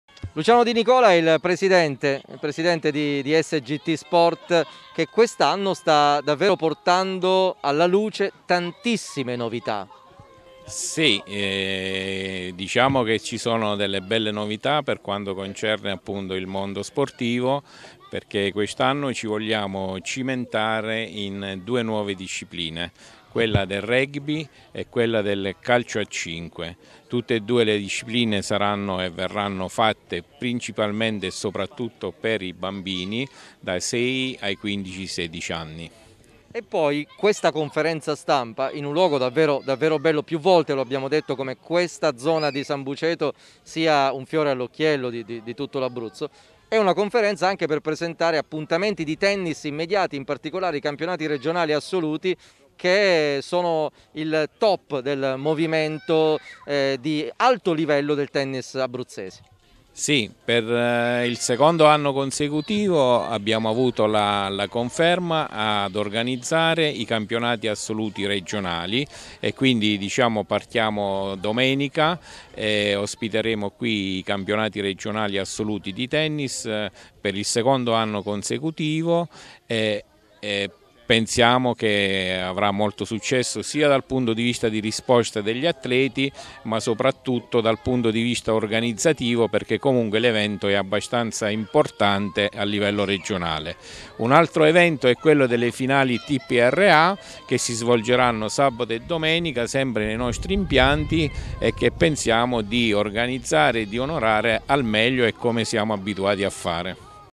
A Sambuceto i campionati regionali di tennis
Da domenica si svolgeranno a Sambuceto i campionati regionali assoluti di tennis. Per la società organizzatrice (la Sgt Sport) è stata anche l’opportunità di illustrare le nuove attività che saranno a breve intraprese nella cittadella dello sport